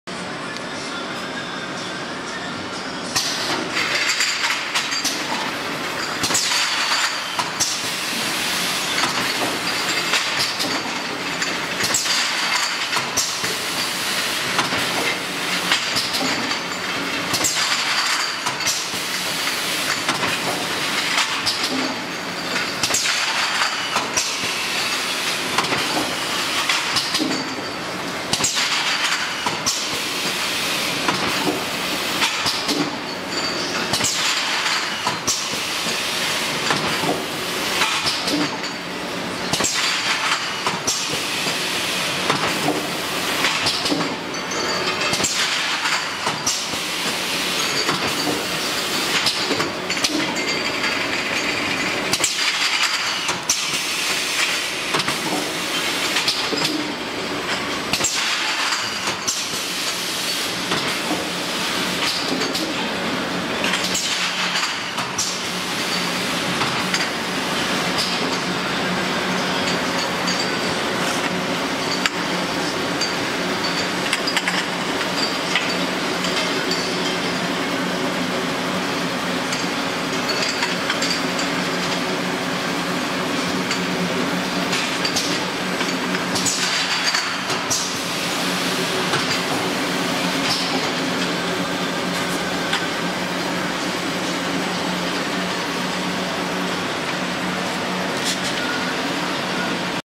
Medicine bottle group bottles sleeve sealing shrink packing machine for clients in Tunisia
One of my clients wanna know our Medicine bottle group bottles sleeve sealing shrink packing machine for clients in Tunisia
00:00 Products feeding
00:08 automated sealing
00:22 hot shrink tunnel
00:37 shrink appearance